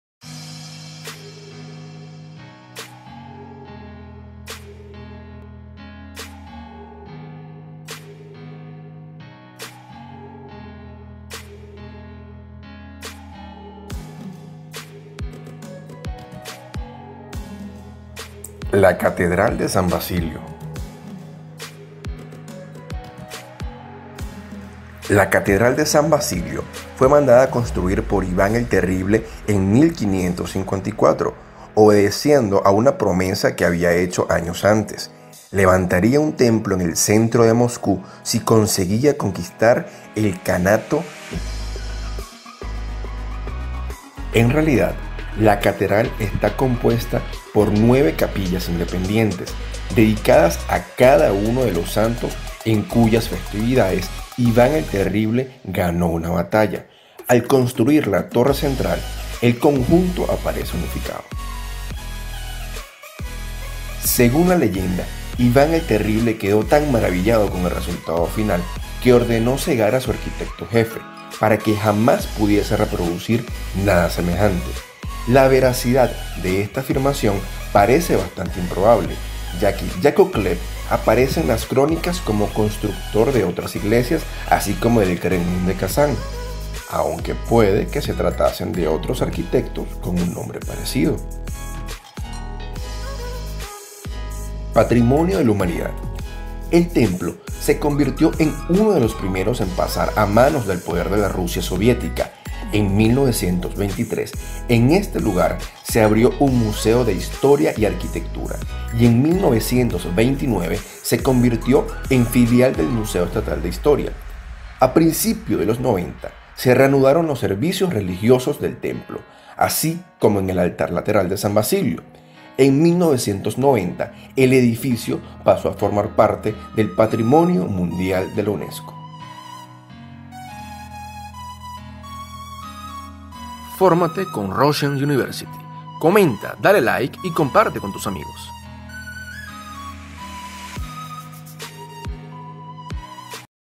LA CATEDRAL DE SAN BASILIO | Estudia en Rusia | Estudios en Rusia
La Catedral de la Intercesión de la Santísima Virgen en el Montículo, más conocida como Catedral de San Basilio, es un templo ortodoxo localizado en la Plaza Roja de la ciudad de Moscú, Rusia.

Si deseas estudiar y viajar a Rusia contáctenos y visite nuestra pagina web: